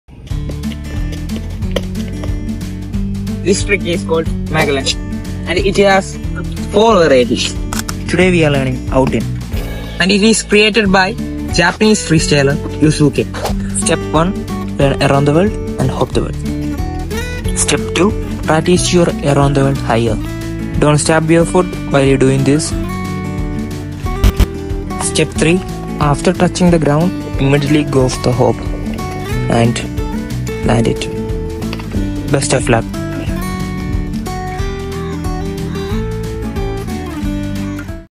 MAGELLAN (Day 1 teaching freestyle football )
Magellan  is medium  range freestyle  trick so i tried to do better in coming  videos so sub it like it comment it

football ) amazon
jersey amazon
pump ) amazon